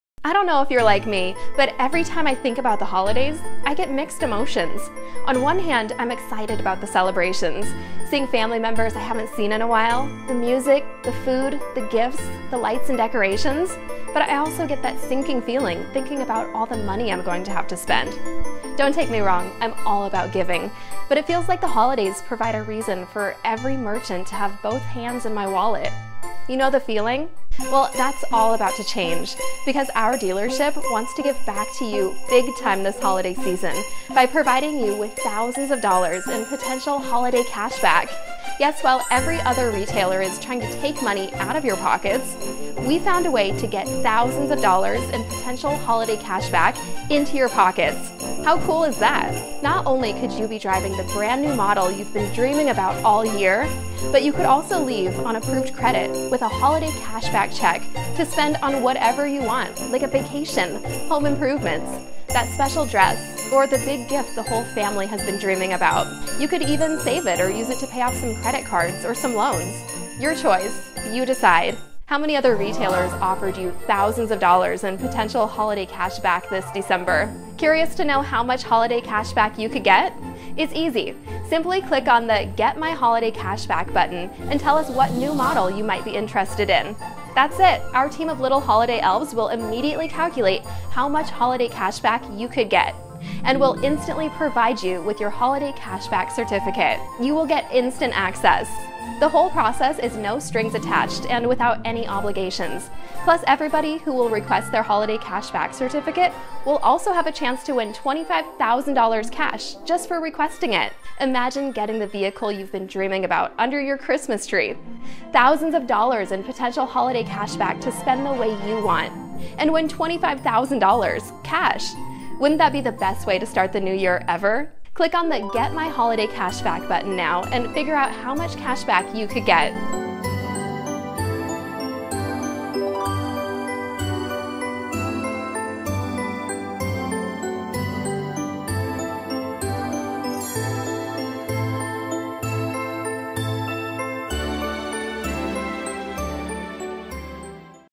Revenberg GM Holiday Cashback Upgrade Event: Get up to $20,000 o.a.c.
I don’t know if you’re like me, but every time I think about the Holidays, I get mixed emotions. On one hand, I’m excited about the celebrations, seeing family members I haven’t seen in a while, the music, the food, the gifts, the lights and decorations; but I also get that sinking feeling thinking about all the money I’m going to have to spend.

Don’t take me wrong, I’m all about giving; but it feels like the Holidays provide a reason for every merchant to have both hands in my wallet. You know the feeling?

Well, that’s all about to change because our dealership wants to give back to you big time this Holiday season by providing you with up to thousands of dollars in Holiday Cashback!

Yes! While every other retailer is trying to take money out of your pockets, we found a way to get up to $20,000 in Holiday Cashback into your pockets. How cool is that?

Not only could you be driving the brand new model you’ve been dreaming about all year, but you could also leave, on approved credit, with a cheque for up to $20,000 to spend on whatever you want like a vacation, home improvements, that special dress, or the big gift for the whole family. You could even save it or use it to pay off some credit cards or some loans. Your choice, you decide.
How many other retailers offered you up to $20,000 in Holiday Cashback this December?

Curious to know how much Holiday Cashback you could get? It’s easy. Simply click on the “Get My Holiday Cashback” button and tell us what new model you might be interested in. That’s it. Our team of little Holiday elves will immediately calculate how much Holiday Cashback you could get and will instantly provide you with your up to $20,000 Holiday Cashback Certificate. You will get instant access!

The whole process is no-strings-attached and without any obligations, plus everybody who will request their Holiday Cash Back certificate will also have a chance to win $25,000 cash just for requesting it!

Imagine getting the vehicle you've been dreaming about under your Christmas tree, up to $20,000 in Holiday Cashback to spend the way you want, and win $25,000 Cash; wouldn’t that be the best way to start the New Year ever?

Click on the “Get My Holiday Cashback” button now and figure out how much cashback you could get!

I wish you a Merry Christmas, Happy Holidays, and a wonderful New Year!